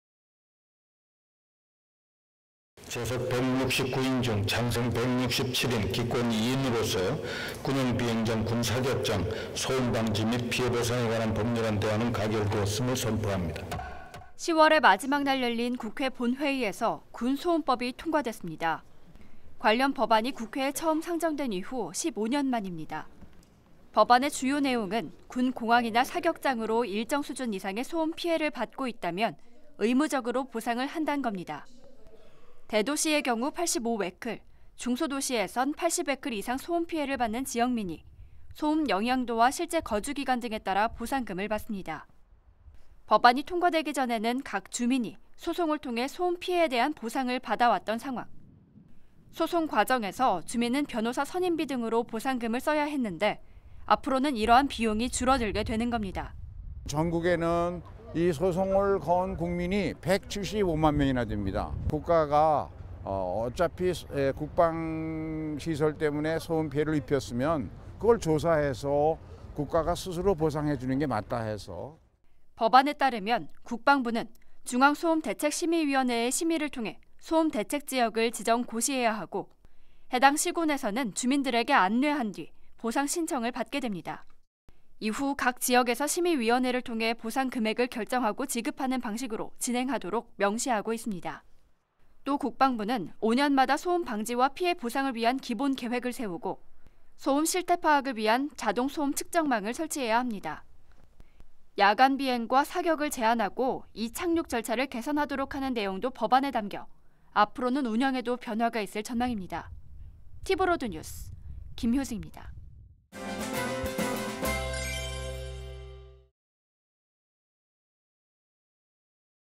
[자막뉴스] ‘군 소음법’ 국회 통과! 이제 소송 없이 피해보상 받자! / 티브로드
수원시민들은 오랫동안 군 공항으로 인한 소음 피해를 호소해
왔습니다. 하지만 피해에 대한 보상은 제대로 이뤄지지 않았는데요.
보상 기준이 명확하지 않았기 때문인데 해결점을 찾게 됐습니
다. 국회에서 군 소음법이 통과됐습니다. 김효승 기자입니다.

취재기자 : 김효승
영상취재 : 노영훈